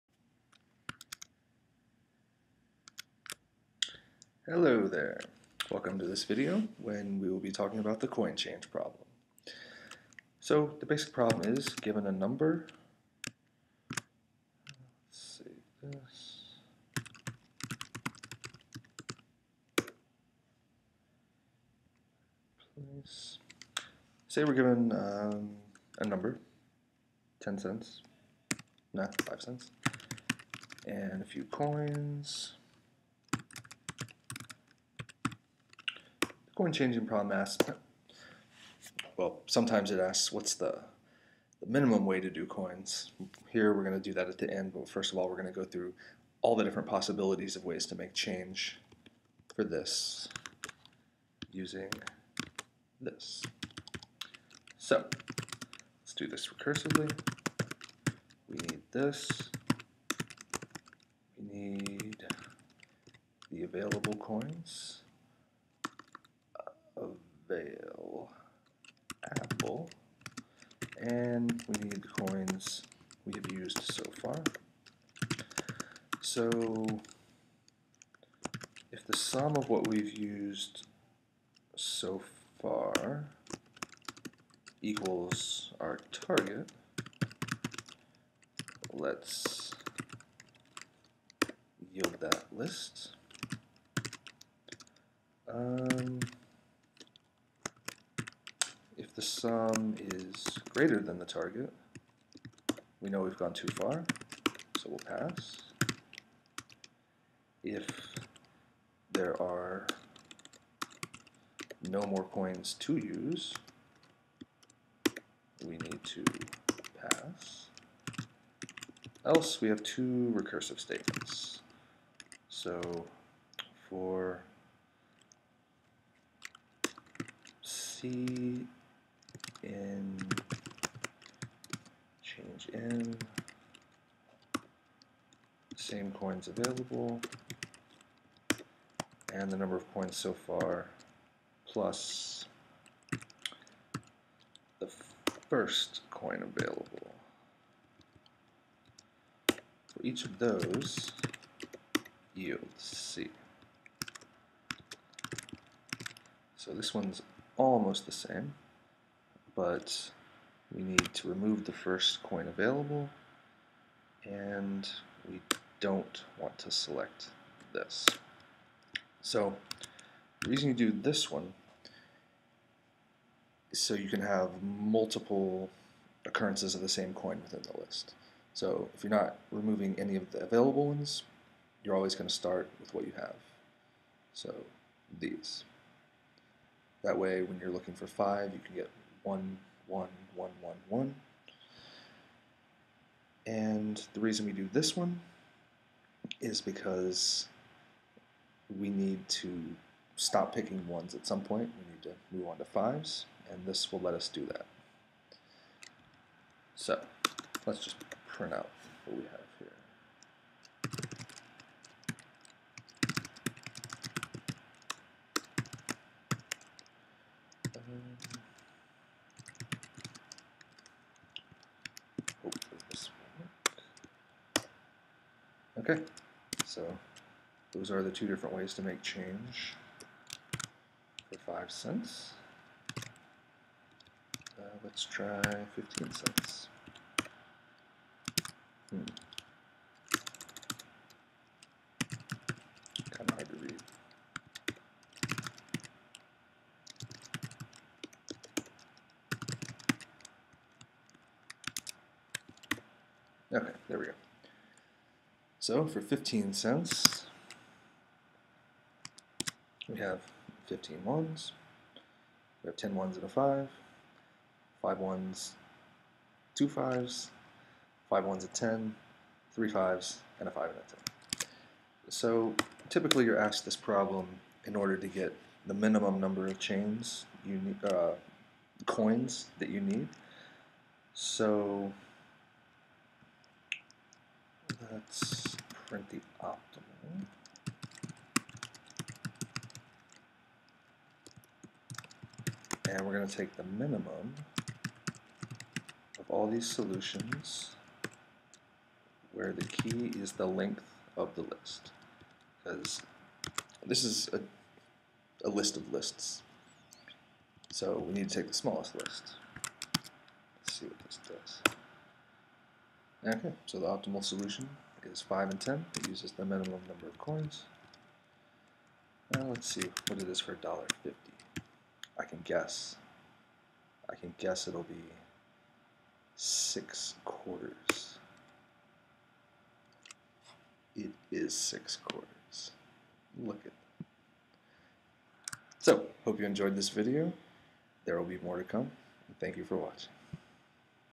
coin changing problem in python
Algorithm for finding the different ways of making change for a given amount using a specified set of coin denominations.